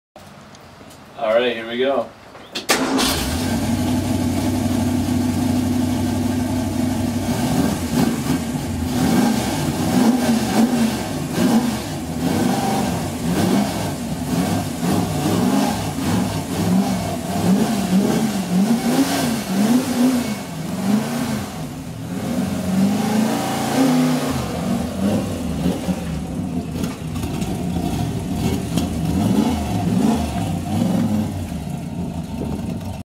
Everett Morrison Cobra
Video of us taking the cobra out of storage in Bemus Point, NY.